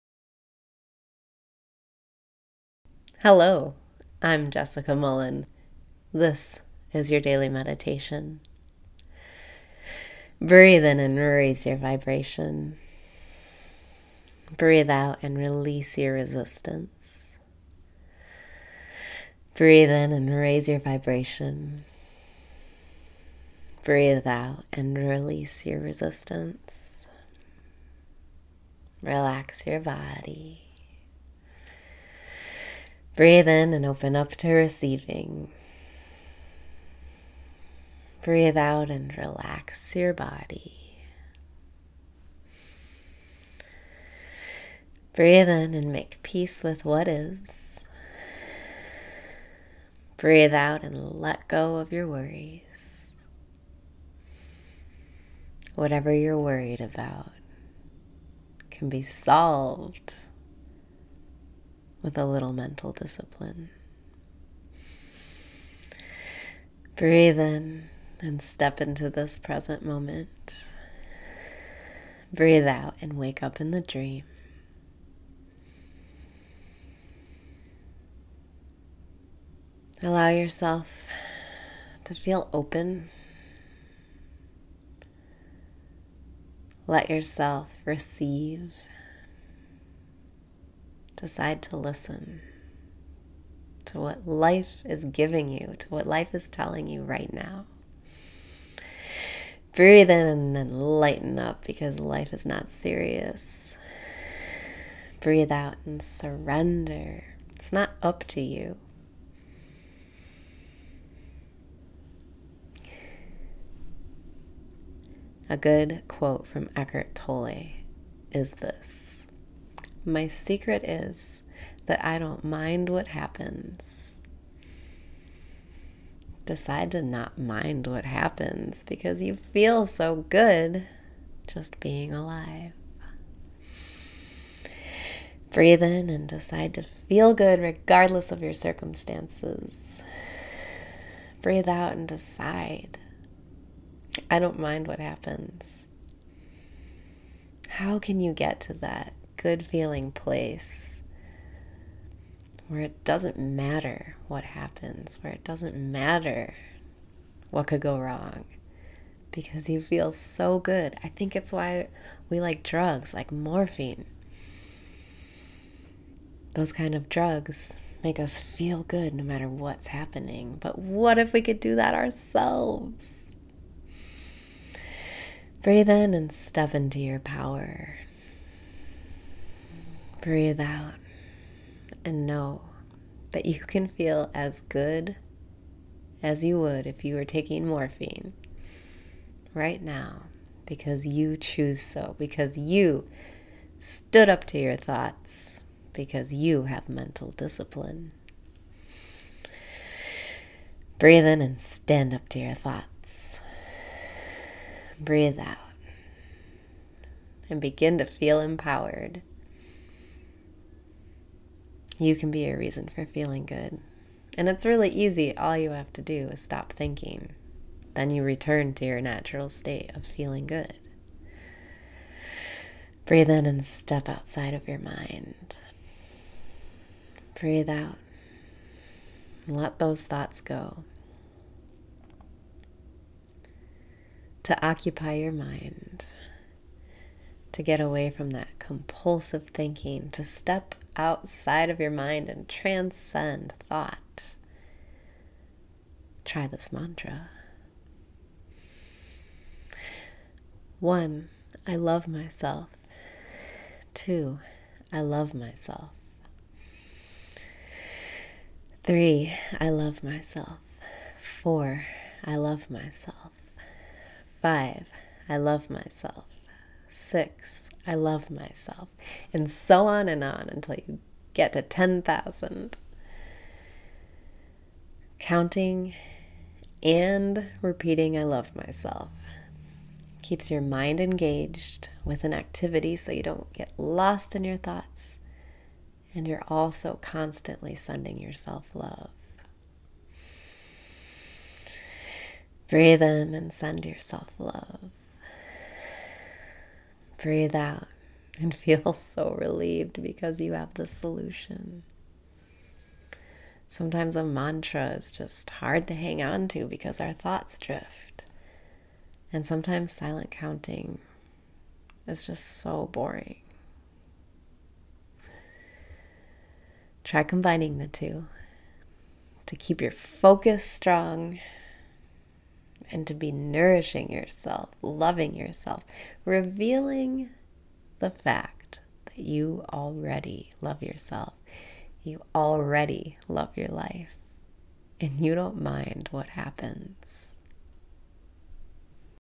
Daily Meditation - Feel as Good as You Would If You Were Taking Morphine
Whatever you’re worried about, can be solved with a little mental discipline!

Breathe in and step into this present moment
Breathe out and wake up in the dream

Allow yourself to feel open. Let yourself receive. Decide to listen to what life is giving you, to what life is telling you right now.

Breathe in and lighten up because life is not serious!
Breathe out and surrender! It’s not up to you!

A good quote from Eckhart Tolle is this: “My secret is, that I don’t mind what happens.” Decide to not mind what happens, because you feel so good just being alive!

Breathe in and decide to feel good regardless of your circumstances!
Breathe out and decide: I don’t mind what happens.

How can you get to that good-feeling place, where it doesn’t matter what happens? Where it doesn’t matter what could go wrong, because you feel so good? I think it’s why we like drugs like morphine. Those kind of drugs make us feel good no matter what’s happening. But what if we could do that ourselves!

Breathe in and step into your power!
Breathe out and know that you can feel as good as you would if you were taking morphine, right now, because you choose so! Because you stood up to your thoughts! Because you have mental discipline!

Breathe in and stand up to your thoughts!
Breathe out and begin to feel empowered!

You can be your reason for feeling good and it’s really easy: all you have to do is stop thinking. Then you return to your natural state of feeling good.

Breathe in and step outside of your mind
Breathe out and let those thoughts go

To occupy your mind, to get away from that compulsive thinking, to step outside of your mind and transcend thought, try this mantra!

1 I love myself 2 I love myself 3 I love myself 4 I love myself 5 I love myself 6 I love myself . . . and so on and on until you get to 10,000! Counting and repeating “I love myself” keeps your mind engaged with an activity so you don’t get lost in your thoughts, and you’re also constantly sending yourself love.

Breathe in and send yourself love
Breathe out and feel so relieved because you have the solution!

Sometimes a mantra is just hard to hang onto because our thoughts drift. And sometimes silent counting is just so boring! Try combining the two to keep your focus strong, and to be nourishing yourself—loving yourself—revealing the fact that you already love yourself! You already love your life! And you don’t mind what happens!